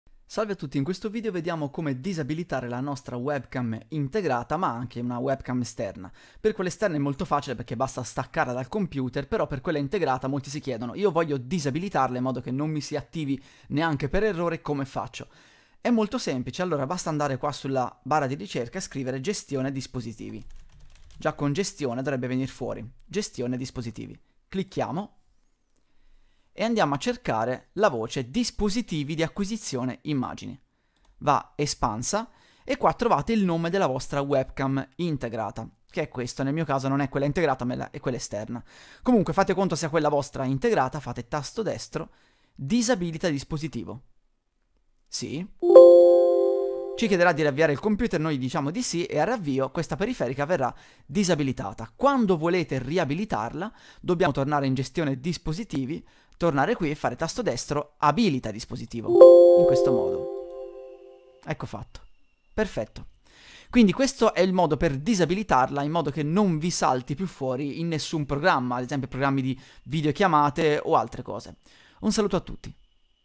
Come disabilitare la webcam integrata del computer su Windows 10
Vediamo come disabilitare una webcam integrata su Windows 10.